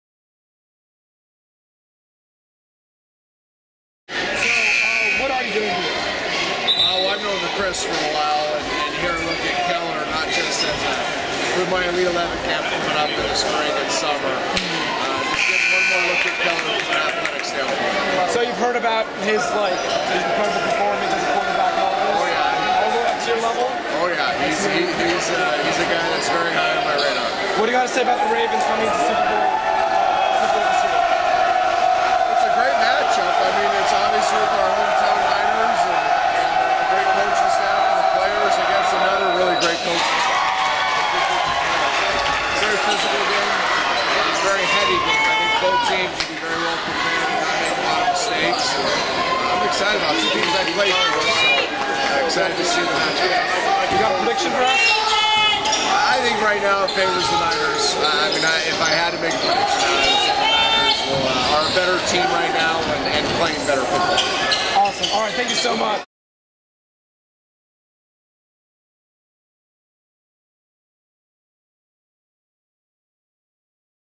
The Viking sits down with Trent Dilfer
Former NFL quarterback Trent Dilfer sits down with The Viking for an interview reasoning his attendance to a Paly boys basketball game. Dilfer was here to watch Keller Chryst, a family friend, and tells us about his Super Bowl XLVII predictions.